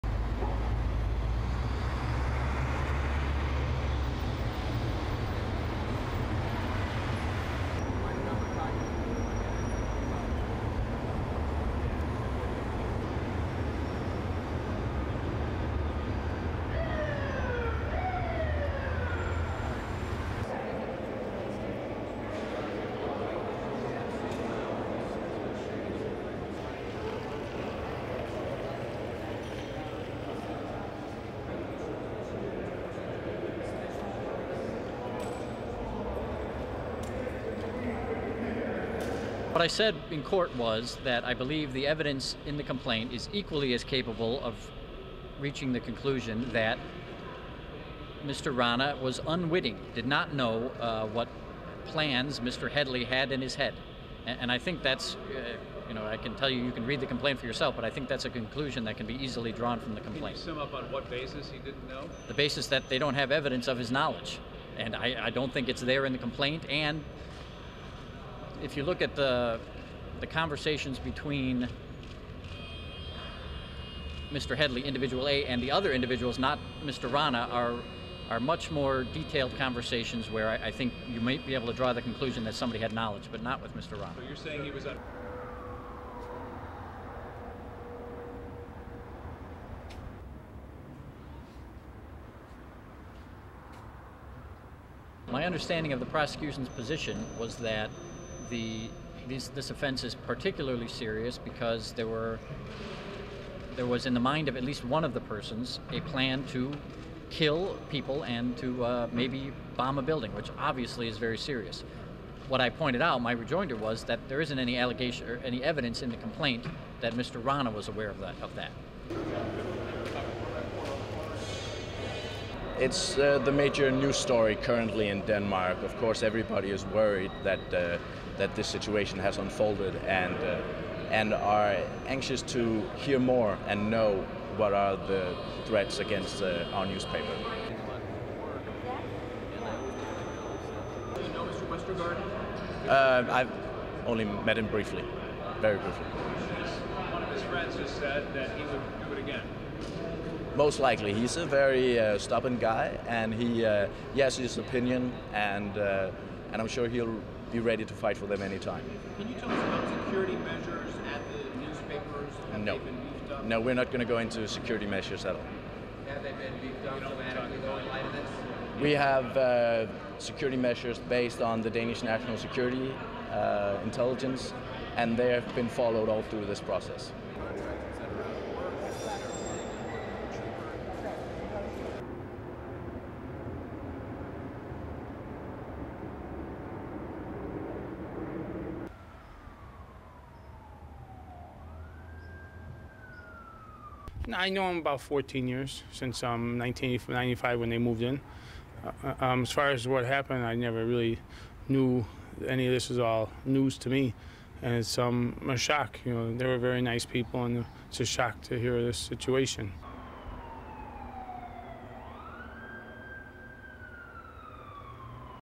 Man accused of plotting attacks against Danish newspaper appears in court
1. Various of courthouse exterior
2. Various court sketches of suspect, Tahawwur Hussain Rana, in court
3. Court sketch of Judge Nan Nolan, pan to sketch of Rana
4. SOUNDBITE (English) Patrick Blegen, Tahawwur Hussain Rana's Attorney:
"What I said in court was that I believe the evidence in the complaint is equally as capable of reaching the conclusion that Mr. Rana was unwitting, did not know what plans Mr. Headly had in his head. And I think that's... you can read the complaint for yourself, but I think that's a conclusion that can be easily drawn from the complaint."
(Question: Can you sum up on what basis he didn't know?)
"The basis that they don't have evidence of his knowledge. And I don't think it's there in the complaint and if you look at the conversations between Mr. Headley, individual A, and the other individuals, not Mr. Rana, are much more detailed conversations where I think you might be able to draw the conclusion that somebody had knowledge, but not with Mr. Rana."
5. Various exteriors of Rana's house
6. SOUNDBITE (English) Patrick Blegen, Tahawwur Hussain Rana's Attorney:
"My understanding of the prosecution's position was that this offence is particularly serious because there was, in the mind of at least one of the persons a plan to kill people and to maybe bomb a building, which obviously is very serious. What I pointed out in my rejoinder was that there isn't any evidence in the complaint that Mr. Rana was aware of that."
7. Close of Kaare Sorensen, New York Correspondent for Danish newspaper, Jyllands-Posten, taking photos
8. SOUNDBITE (English) Kaare Sorensen, New York Correspondent for 'Jyllands-Posten':
"It's the major news story currently in Denmark; of course everybody is worried that this situation has unfolded and are anxious to hear more and know what are the threats against our newspaper."
9. Court sketch of Rana
10. SOUNDBITE (English) Kaare Sorensen, New York Correspondent for 'Jyllands-Posten'
(Question: Do you know Mr. (Kurt) Westergaard (Danish cartoonist who created the controversial cartoon of the Prophet Muhammad)?)
"I have only met him briefly, very briefly."
(Question: One of his friends has said that he would do it again?)
"Most likely, he is a very stubborn guy and he has his opinion and I'm sure he would be ready to fight for them at any time."
(Question: Can you tell us about security measures at the newspaper?)
"No, we're not going to go into security measures at all."
(Question: Are they beefed up?)
"We have security measures based on the Danish National Security Intelligence, and they have been followed all through this process."
11. Court sketch of Rana, pan to judge
12. Zoom out of Rana's house
13. Pan of neighbourhood
14. SOUNDBITE (English) Nathan Schein, Neighbour:
"I know him about 14 years, since 1995 when they moved in.  As far as what happened I never really knew any of this, it's all news to me and it's a shock; they were very nice people and it's a shock to hear this situation."
15.  Pan of Rana's house
STORYLINE
Prosecutors urged a federal judge on Wednesday to deny bail to a Chicago man accused of plotting an attack on a Danish newspaper that published cartoons depicting the Prophet Muhammad, angering the Muslim world.
Assistant US Attorney Daniel Collins told federal Magistrate Judge Nan Nolan that the suspect, Tahawwur Hussain Rana, 48, was a danger to the community and a flight risk and therefore should not be released on bail.
Rana's lawyer, meanwhile, argued that his client could have been the innocent dupe of an alleged co-conspirator.